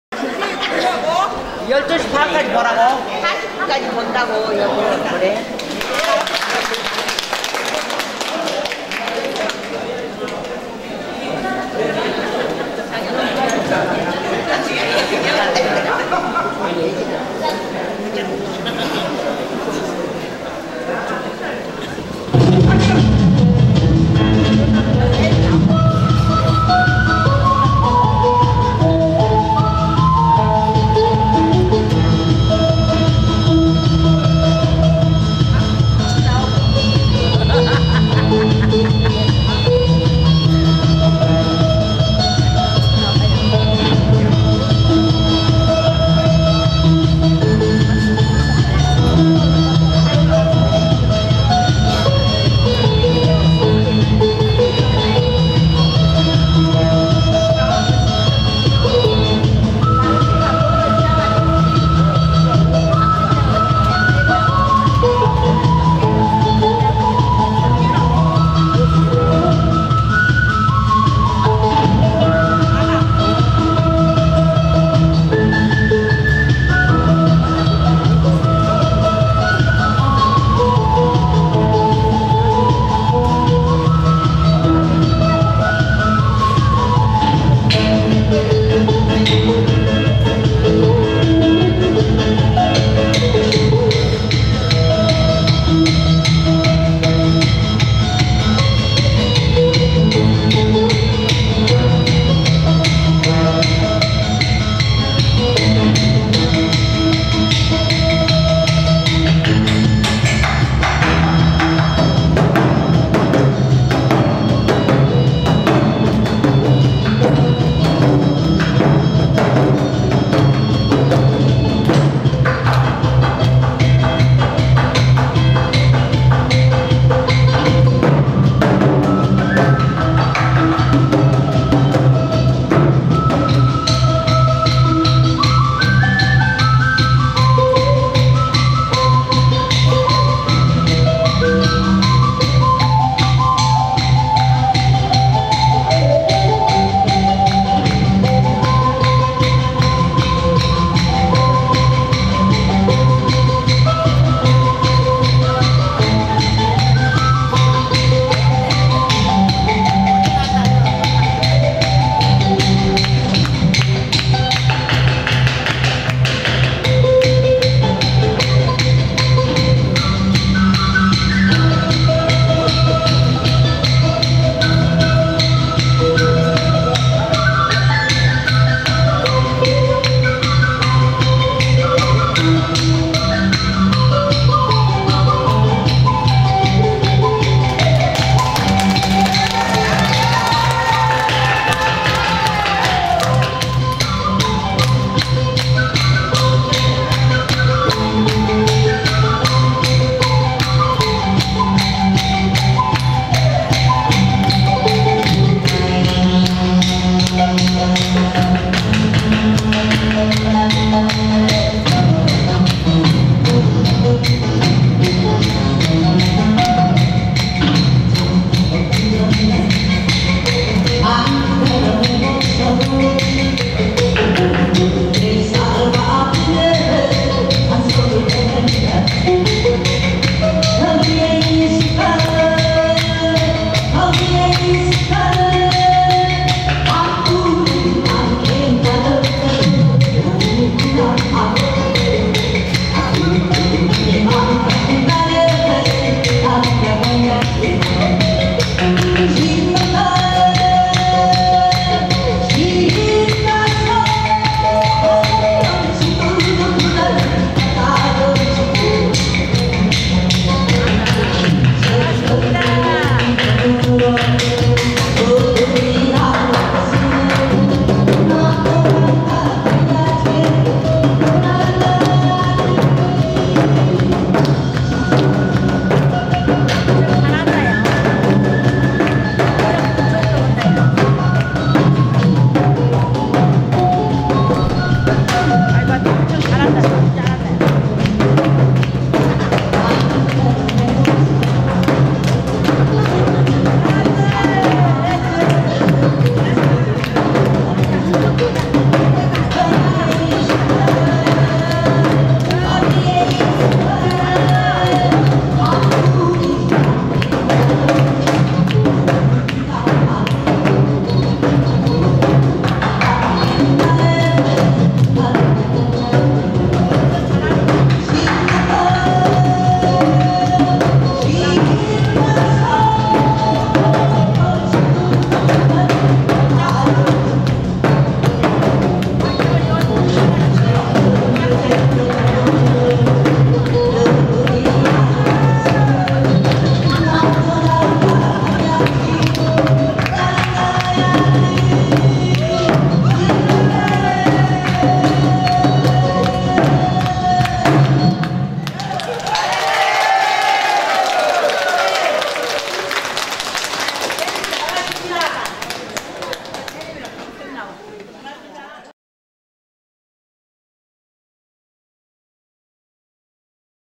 자수정동굴 오북학춤WMV
설명 : 한국정통 오북학춤 정종수 울산 언양온천단지 자수정동굴 1일 6회공연